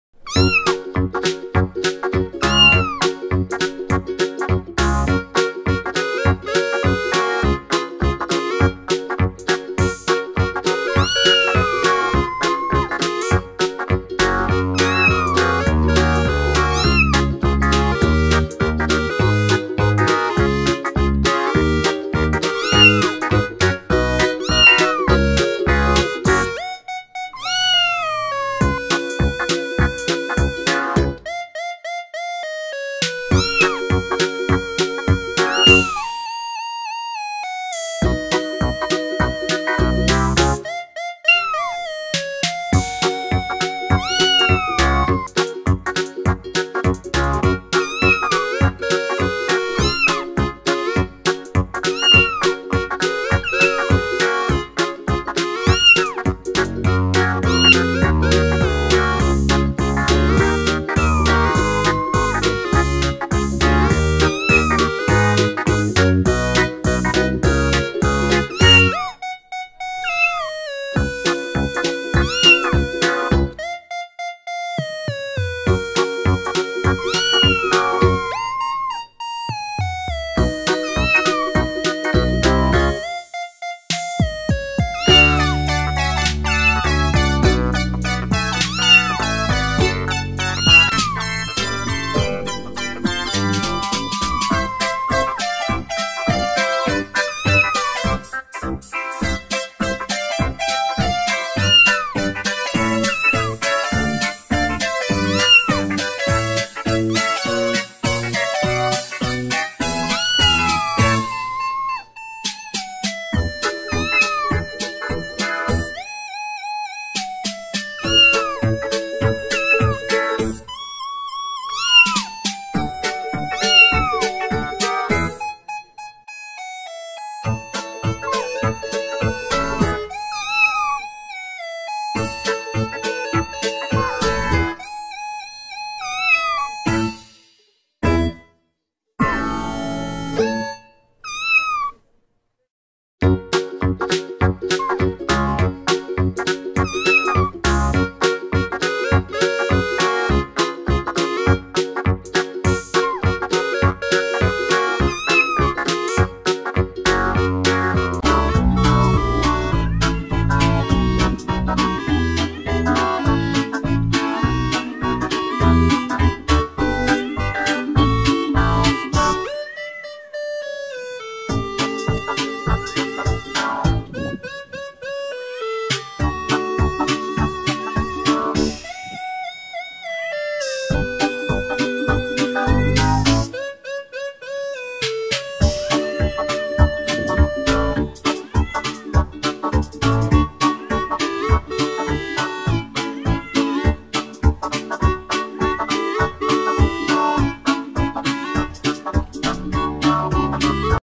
Mama Miau Meong
Kucing yang lucu, mereka berkomunikasi secara vokal juga jadi alasan kenapa kucing lucu. Banyak kucing yang bersuara menggemaskan saat meminta sesuatu. @Infoterkini-ind